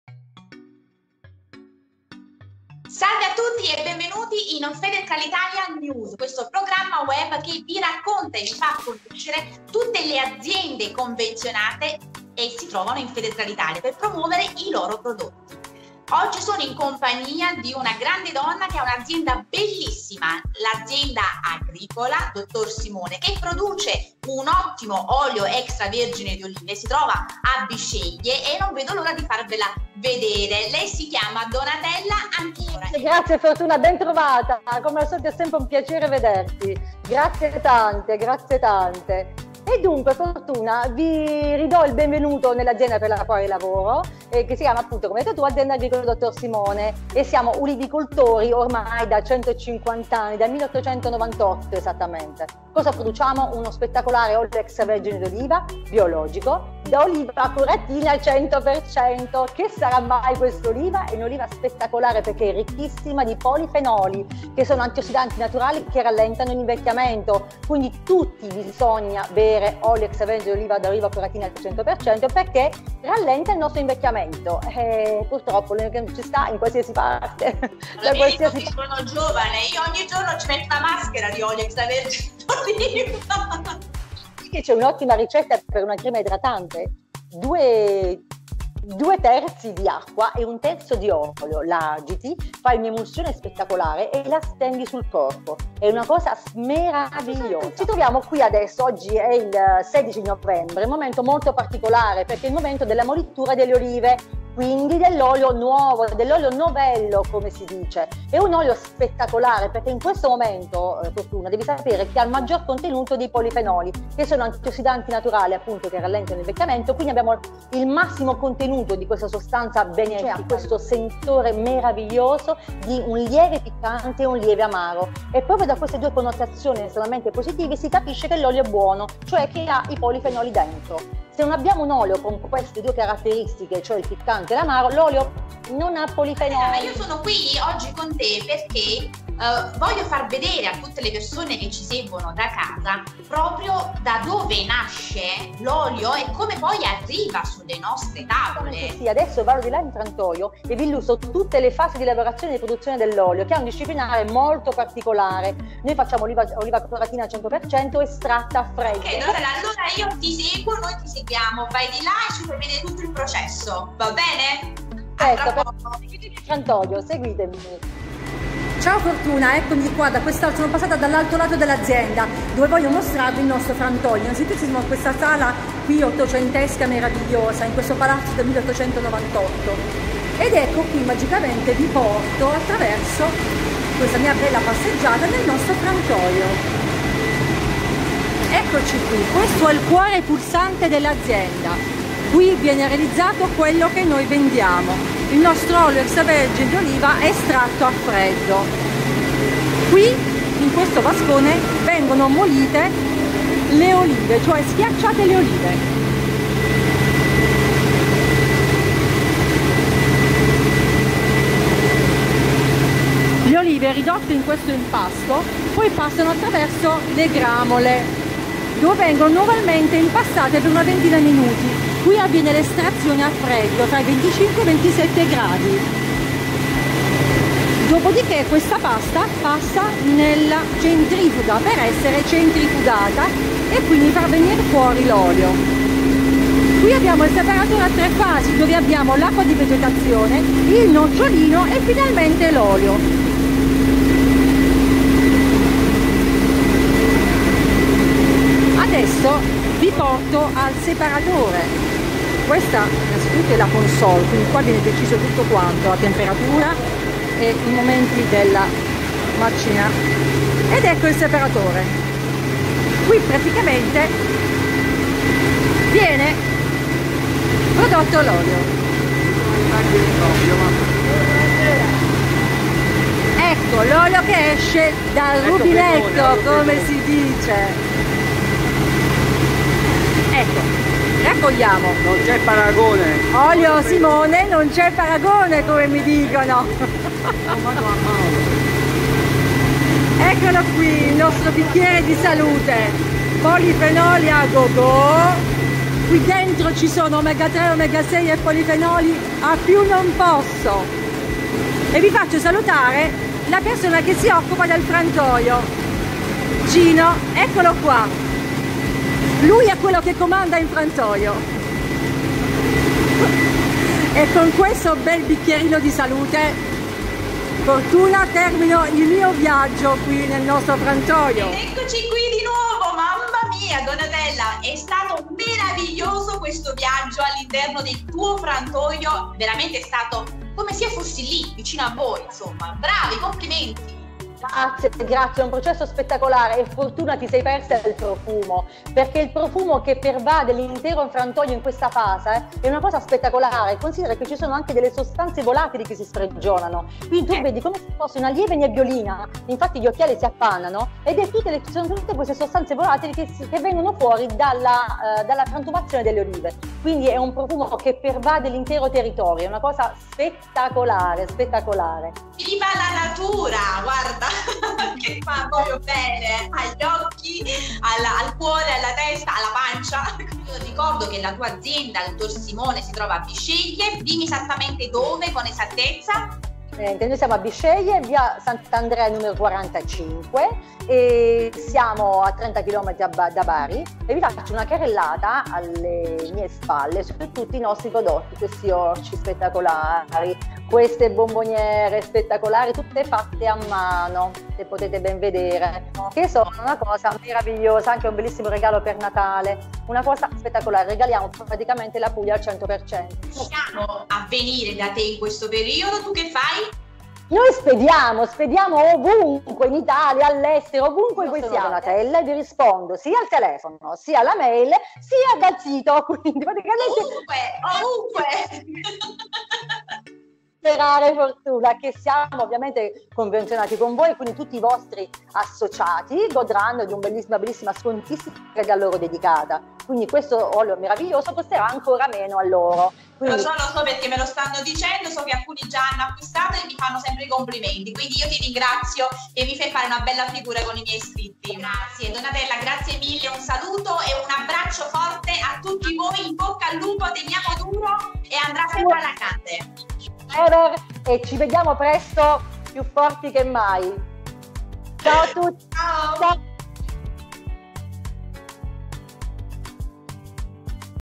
Feder Cral Italia News presenta Azienda Agricola Dottor Simone - Molitura Olive
L' Azienda agricola dottor Simone di Bisceglie è un convenzionato Feder Cral Italia.
Oggi vi presentiamo come è il processo delicato della molitura olive nel loro frantoio.
Produzione di Olio Extra Vergine D'Oliva.

Promuovi anche tu la tua azienda, fai una video intervista con noi, contattaci al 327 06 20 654 Promuovi i tuoi prodotti.